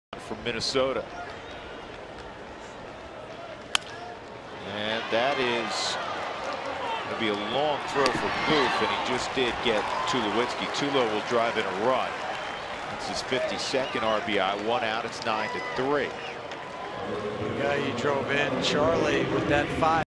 MIN@COL: Tulo's groundout knocks in Blackmon
7/12/14: Troy Tulowitzki collects an RBI as Charlie Blackmon crosses the plate on his groundout to third in the 9th inning